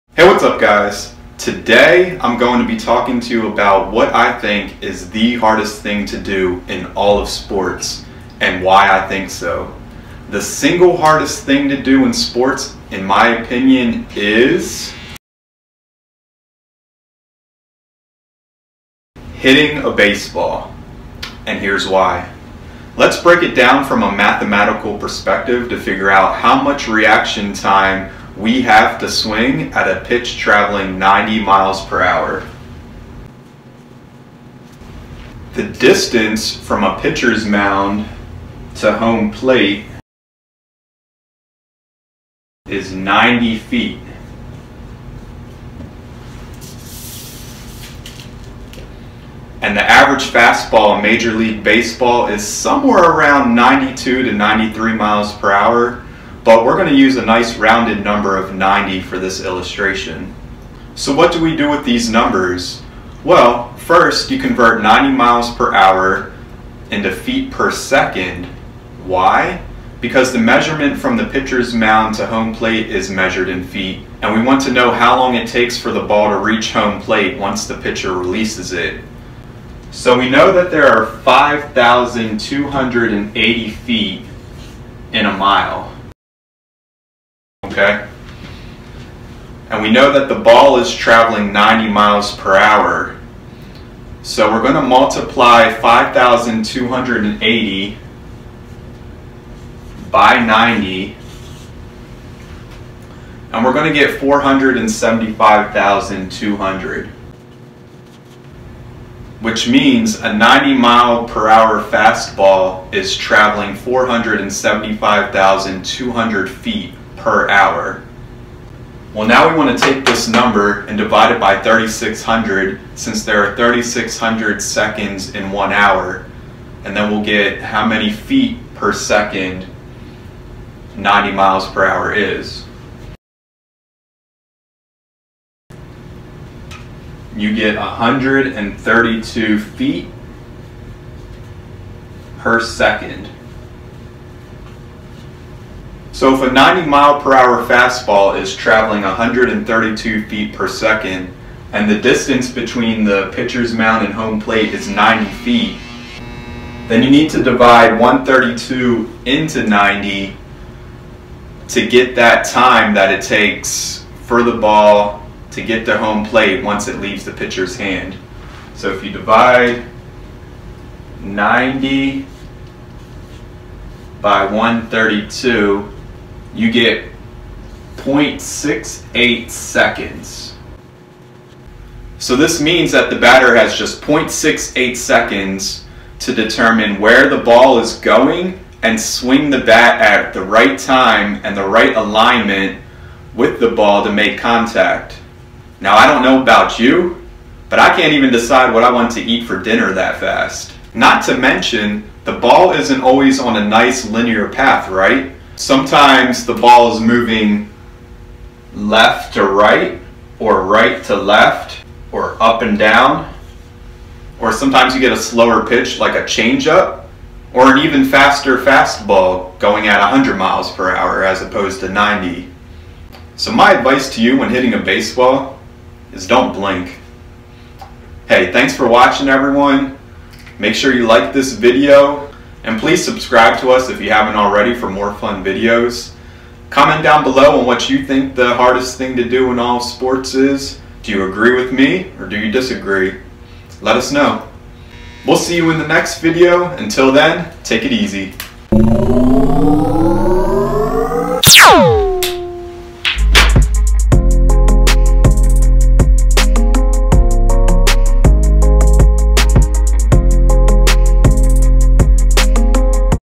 The Single Hardest Thing To Do In All Sports
Fitness Specialist Jimmy Allen breaks down from a mathematical perspective what he believes to be the single hardest thing to do in all of sports.

Agree or disagree?  Drop a comment on what you believe is the hardest thing to do in sports.

Don't forget we're on Facebook! Meade CYS